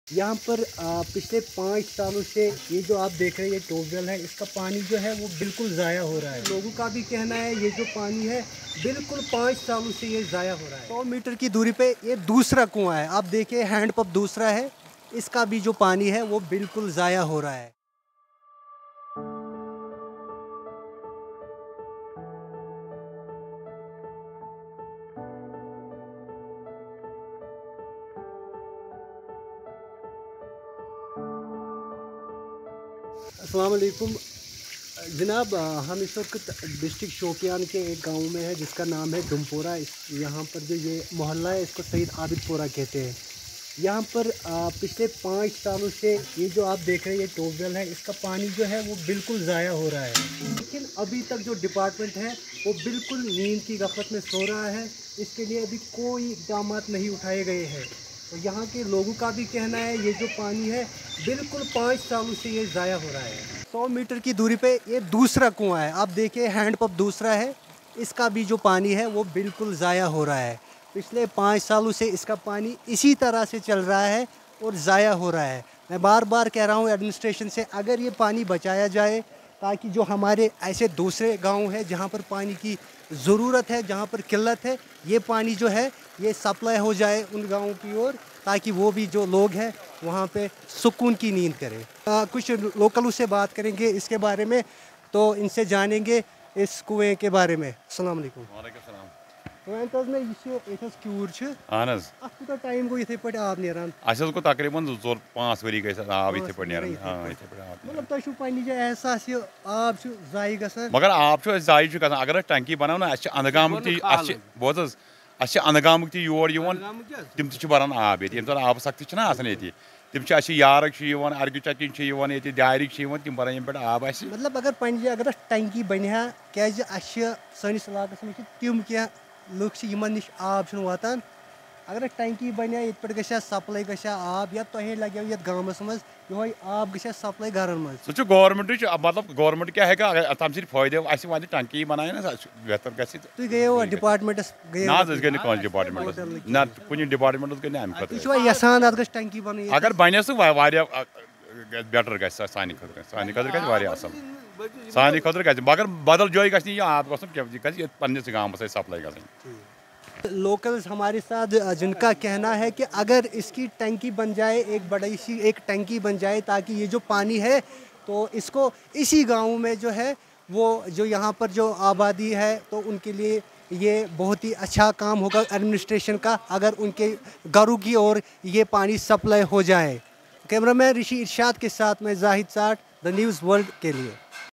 This village in Shopian has been witness to wasting of water for the past six years
This village in Shopian has been wasting water for the past six years.